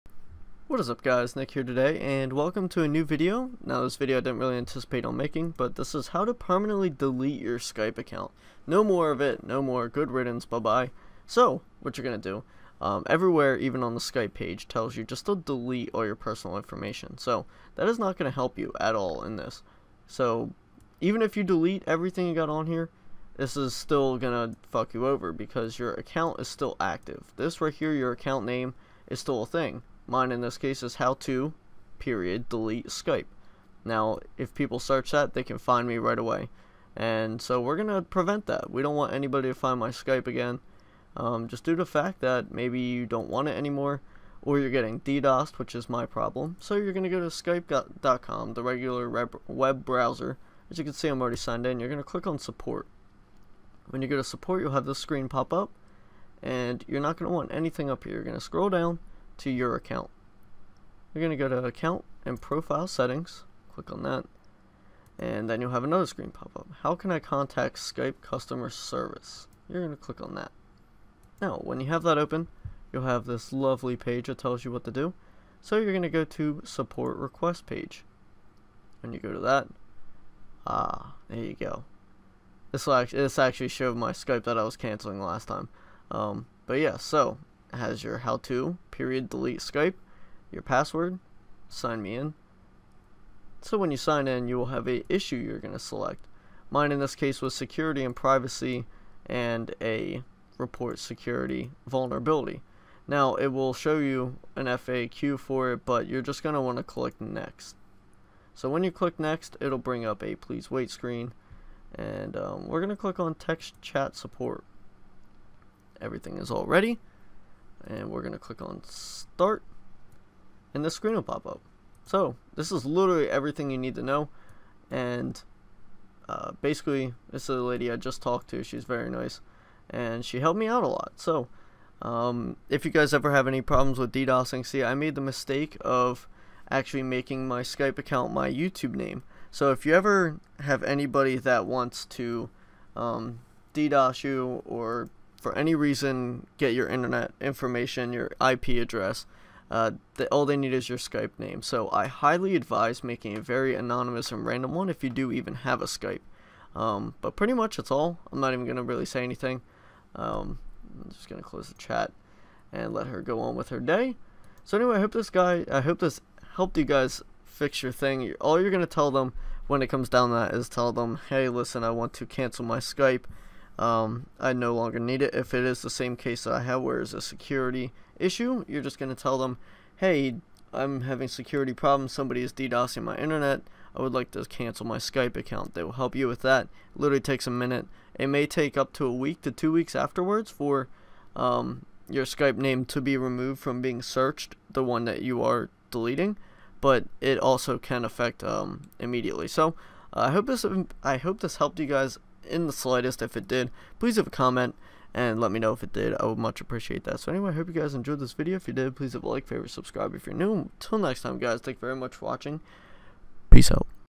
How to Permanently Delete your Skype Account 2017
Hope this helped you guys out if you were struggling finding out how to get rid of skype!